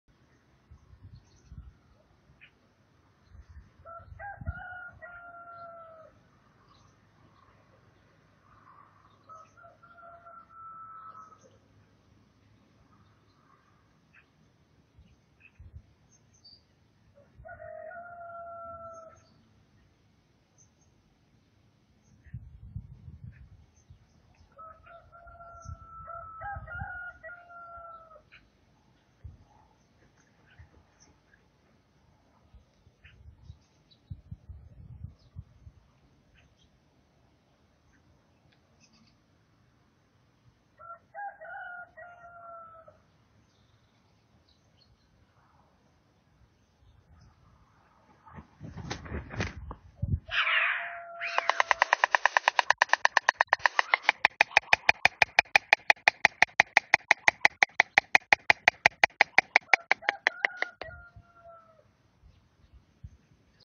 Долгий прощальный стук аиста. 30 августа 2022 в 6:12
Всю ночь с 29 на 30 августа  Степа провел на столбе. Утром в сильном тумане его контуры чуть угадывались в далеке.
В 6-11-50 под крик петухов   слетел со столба и сделав, прощальный круг 0:47  прилетел на гнездо.
Долго и с выражением пощелкал всей округе и всем зрителям. Прощается с нами до следующей весны.